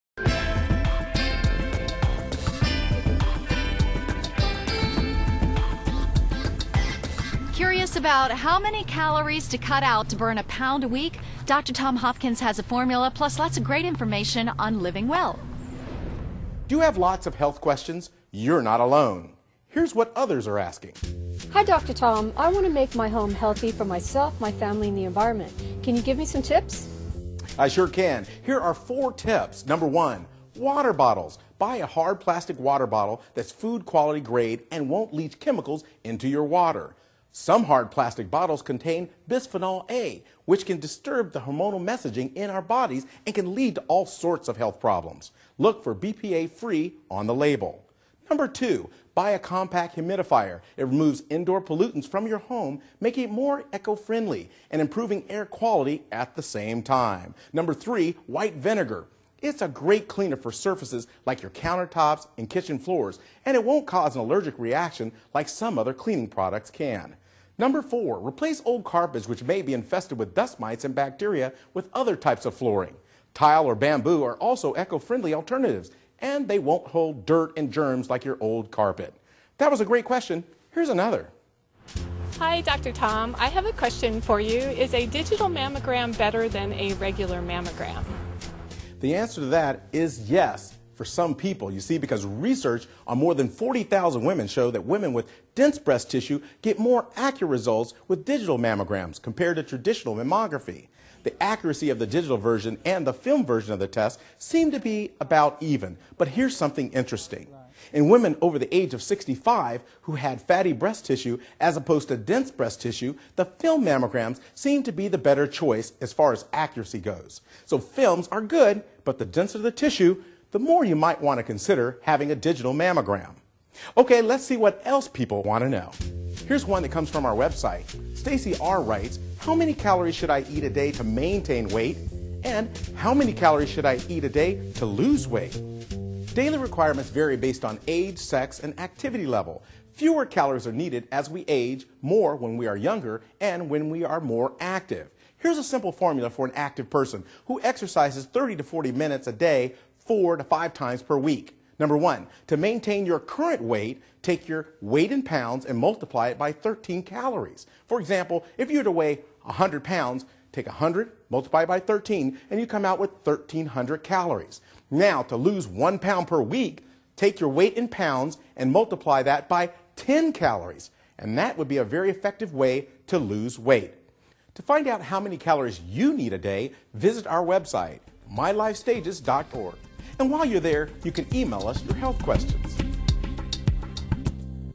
Ask the Doctor | How to keep your home health, and is digital mammography better?
Dr. Tom Hopkins of Sutter Independent Physicians gives four tips to keep you home healthier. Tips on how to keep your home health, digital mammography and how many calories to eat to lose weight.